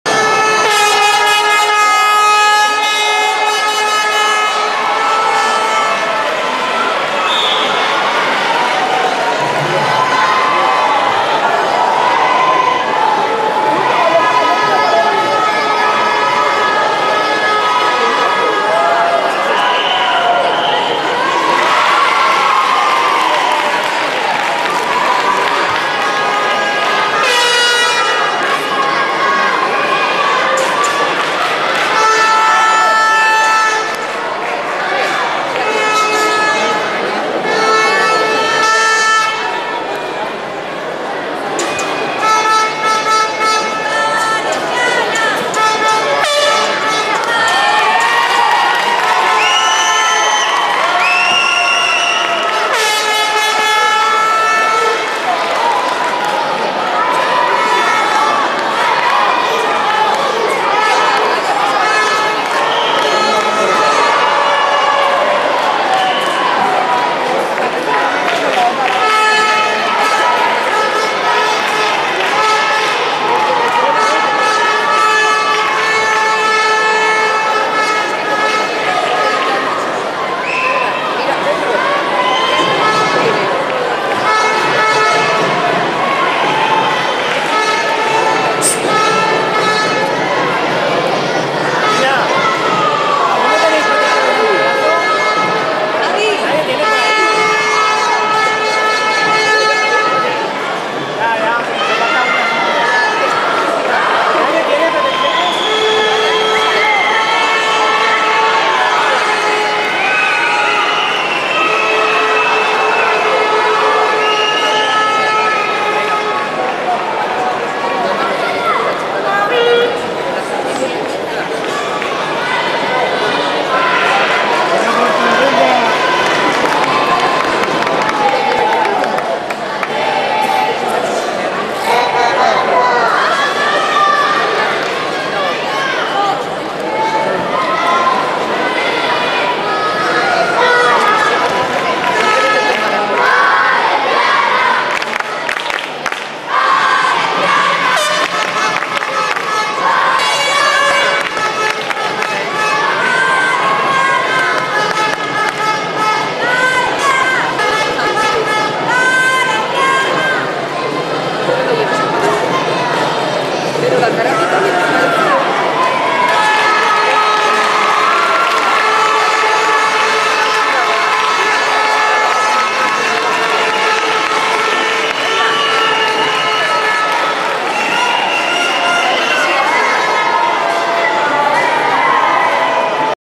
2009.04.25 - Cpto. España - Pinan Nidan vs Heian Sandan
2ª Ronda Clasificatoria Kata Campeonato Karate Infantil España 2009